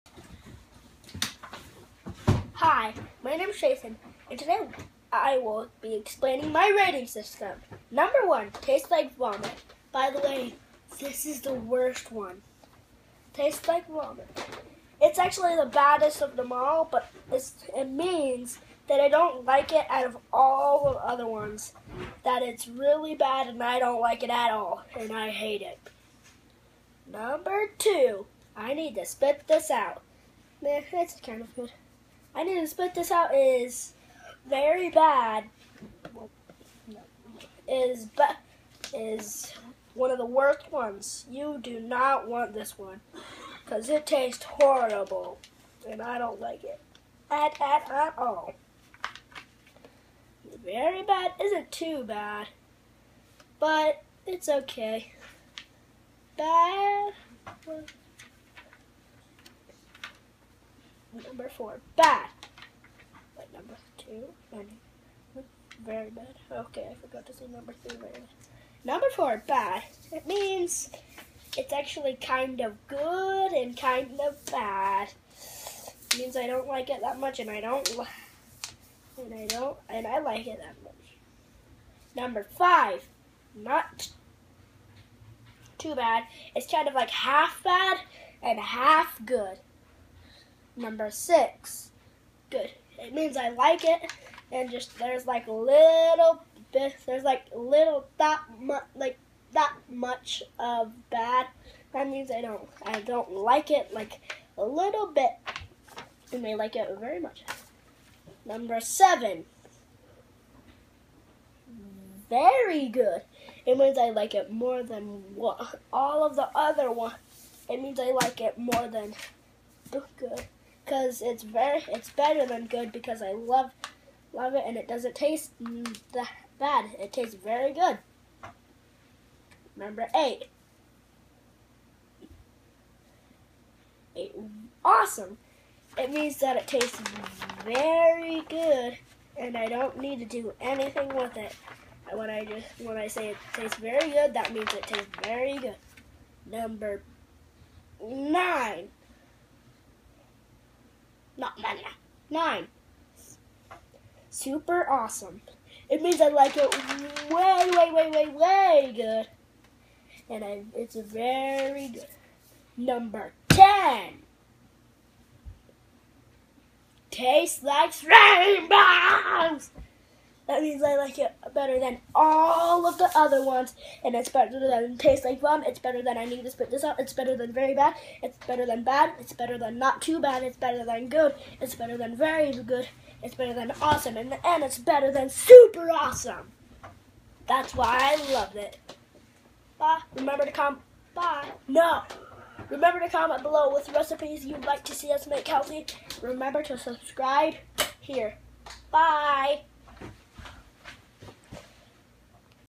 Tresen explains his rating system
want to know what Tresen means when he says something is not too bad? or just how good is SUPER awesome? Today Tresen explains these and more.